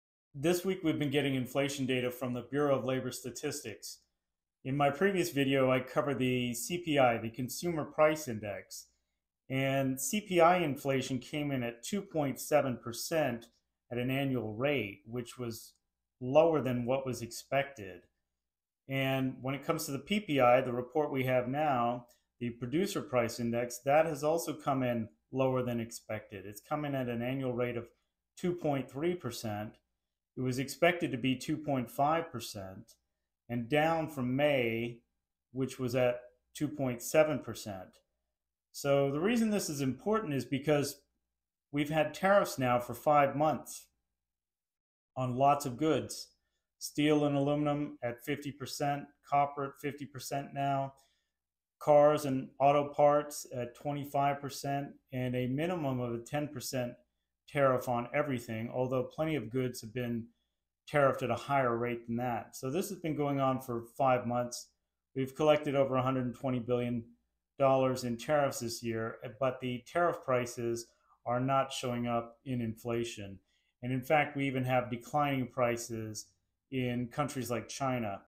Inflation Data: Unexpected Trends & Tariff Impact
Our analysis of recent inflation data from the Bureau of Labor Statistics reveals surprising trends. We unpack the latest CPI and PPI figures, exploring why tariff impacts aren't reflected in inflation. Discover key insights into economic shifts and price declines in China.